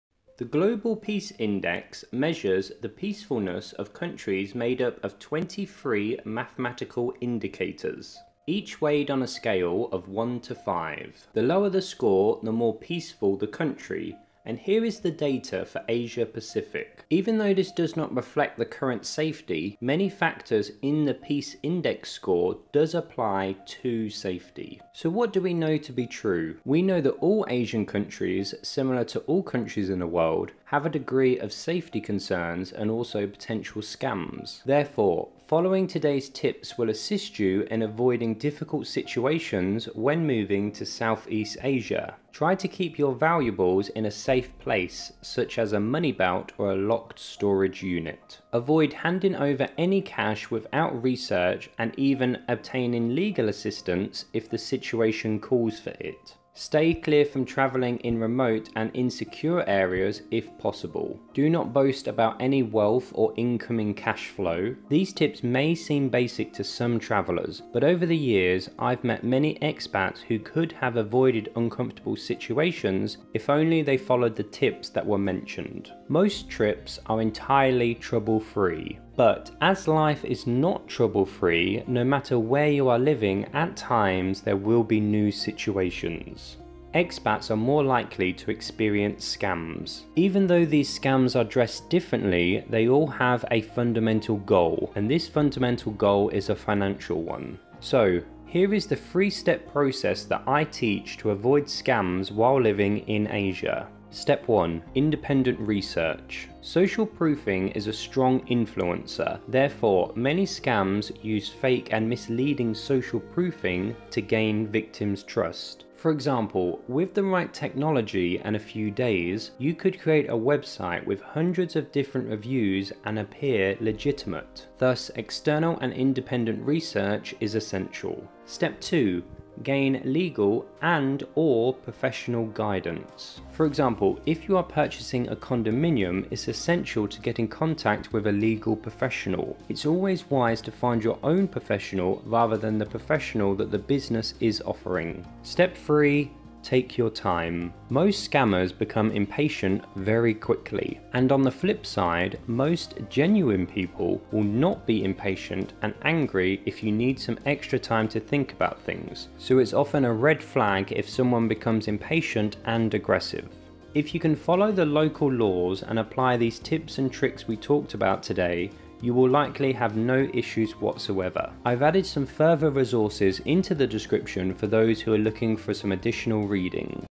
SCAMS To Avoid In Southeast Asia (Copyright Free Content)
Safety In Asia: When looking at safety in Asia, there are many categories such as food safety in Asia, solo traveling safety in Asia, etc. Today we will be addressing two main safety concerns in Asia. In this video, we won't be going over common safety problems and scams in Asia as we have covered this previously. Instead, we will explore different tips to help you stay safe in Asia while traveling. Safety In Asia

Extra Info:  Safety in Asia

🦉Social: Nope, Only Youtube.